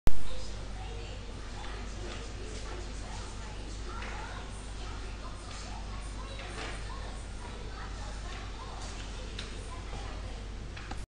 Paramecium Video
This is a video of a paramecium culture taken with a microscope and AverVision document camera.